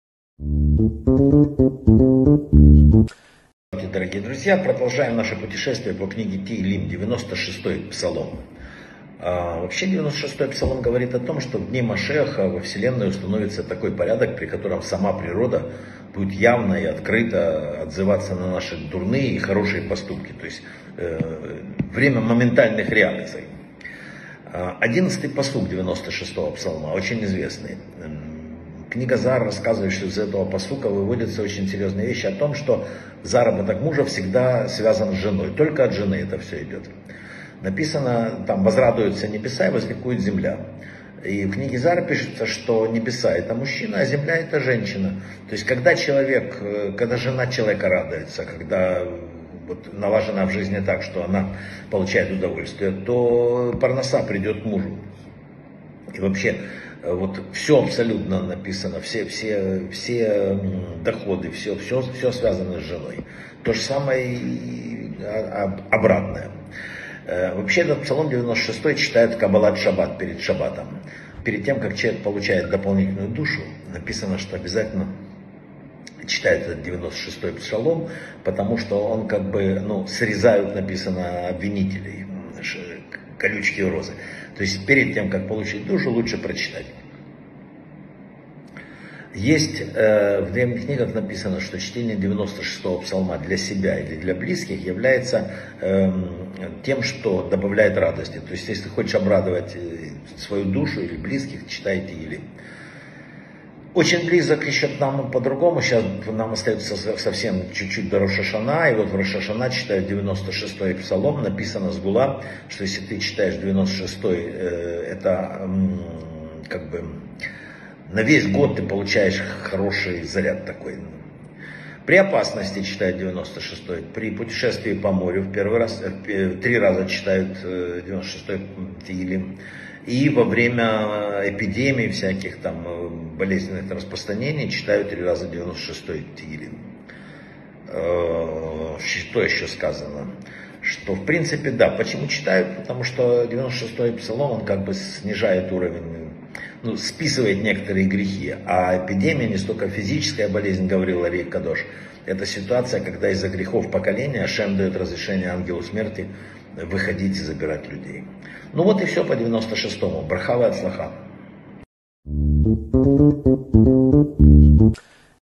📗 Псалмы Давида. Псалом 96. Духовный отклик в дни Машиаха. Жена и заработок мужа | Вадим Рабинович
Какой порядок будет установлен в дни Машиаха и что это будет означать для человека?
Чем особо известна одиннадцатая строка этого Псалма? Какой закон о финансовом благополучии семьи из нее выводится?
"И возрадуются небеса, и возликует земля" - узнаем иносказательный смысл этой фразы.
Почему этот Псалом читается перед приходом Шабата, когда к человеку приходит дополнительная душа?
Что может сделать тот, кто хочет порадовать свою душу или души своих близких и привнести позитивное духовное влияние?
Особое свойство этого Псалма перед Рош аШана - что он поможет получить на весь год?
Защита от опасности, во время морских путешествий и во время эпидемий. Какими духовными качествами обладает этот Псалом?